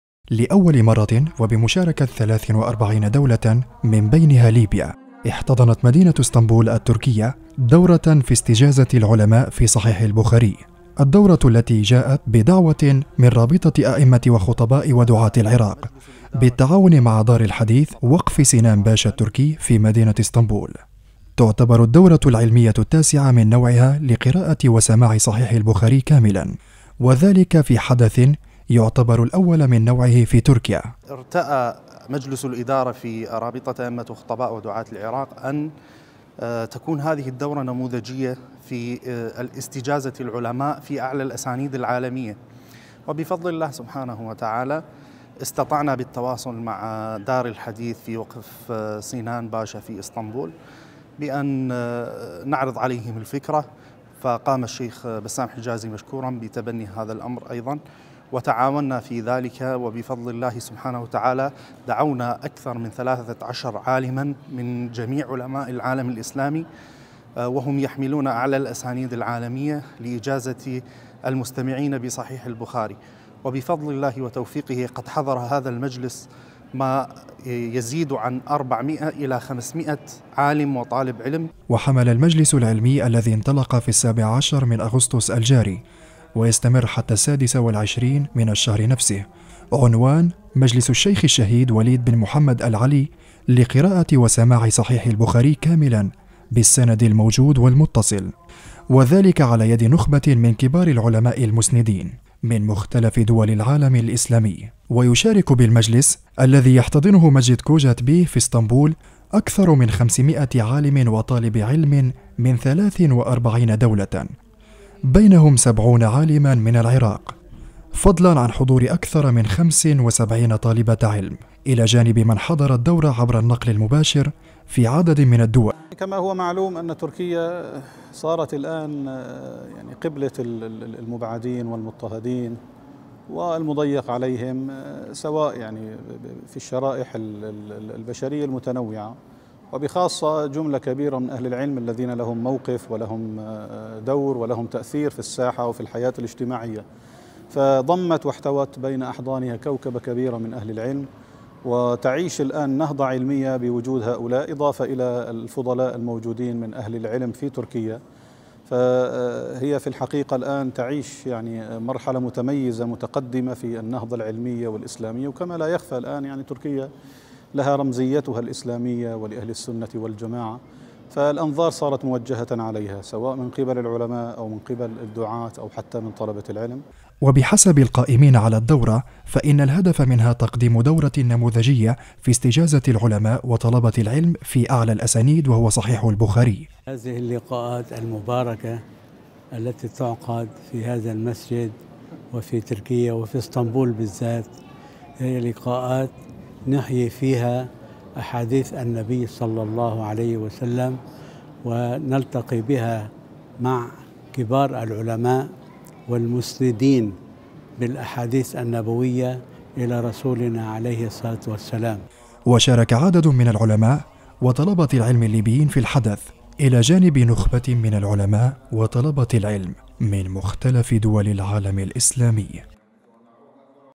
تقرير | اسطنبول: دورة علمية لسماع وقراءة صحيح البخاري. | 22 - 8 - 2019
ــــــــــــــــــــــــــــــــــــــــــــــــــــــــــــــــــــــــــ
ــــــــــــــــــــــــــــــــــــــــــــــــــــــــــــــــــــــــــ
يمكنكم تحميل تطبيقنا TANASUH TV على الهواتف الذكية:
ــــــــــــــــــــــــــــــــــــــــــــــــــــــــــــــــــــــــــ
التناصح